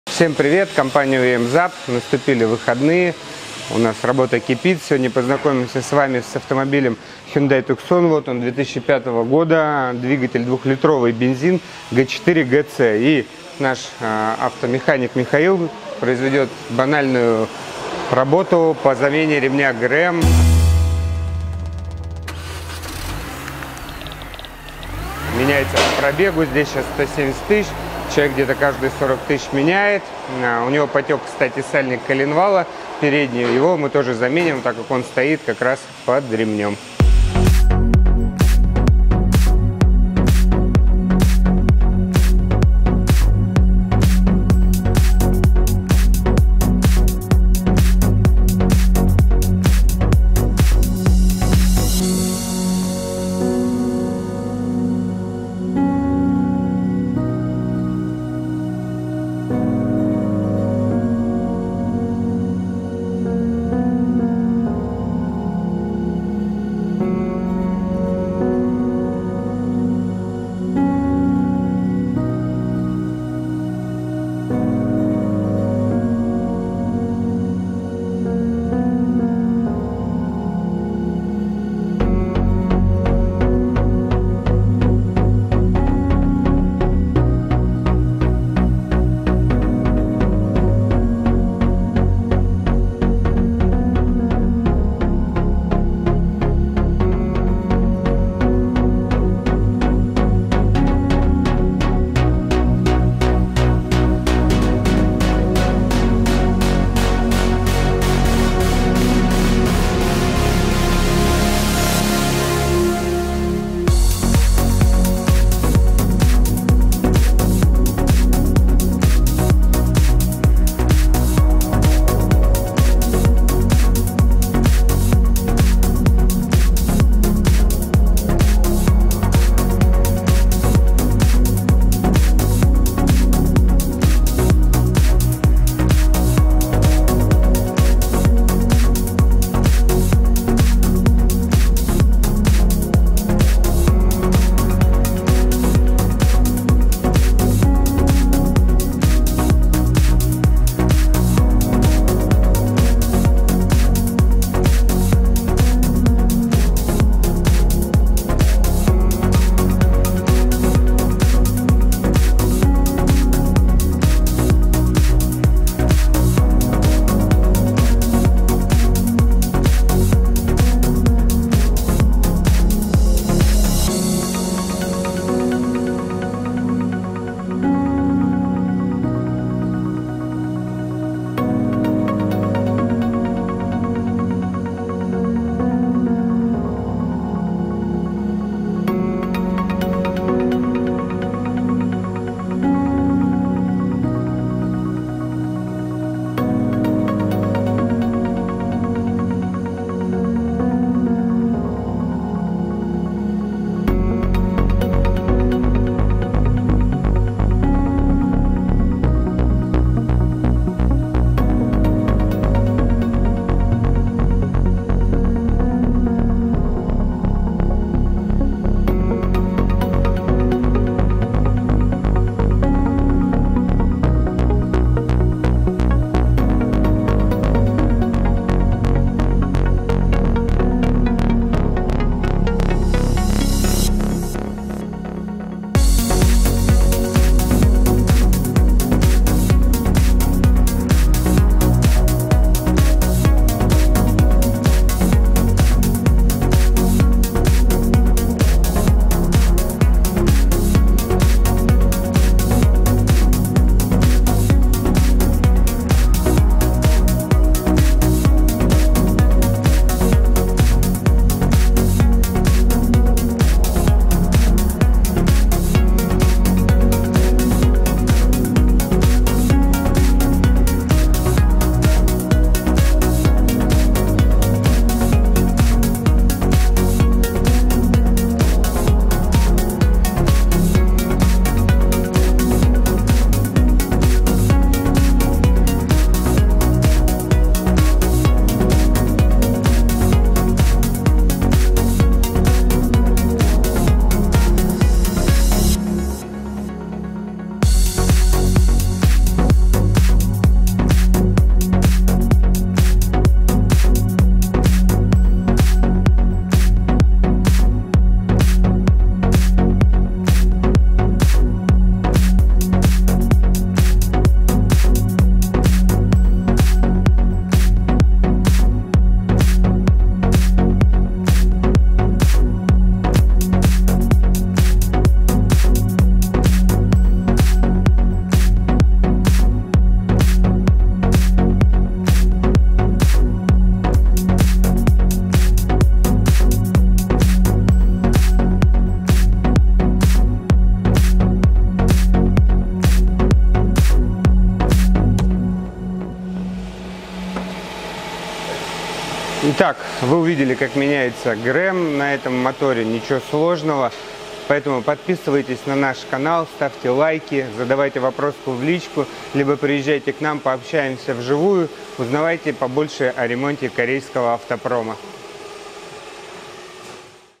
Банальная замена ГРМ Хендай Туксон G4GC
Находимся по адресам:
👇👇👇👇👇👇👇👇👇ЖМИ КНОПКУ "ЕЩЁ"👇👇👇👇👇👇👇👇👇

Север
🕘🔛🕗 График работы: с 9 до 20 без выходных.

Такая ли банальная эта операция по замене ремня ГРМ на автомобиле Hyundai Tucson с двигателем G4GC? Конечно банальная, если вы обладаете хорошим знанием матчасти и у вас есть опыт проведения подобных работ! Да! И еще, если у вас есть определенный набор инструмента 😊  Ну а если у вас чего-то не хватает, тогда не скупитесь и приезжайте к нам. Для наших мастеров это действительно банальная и ежедневная работа.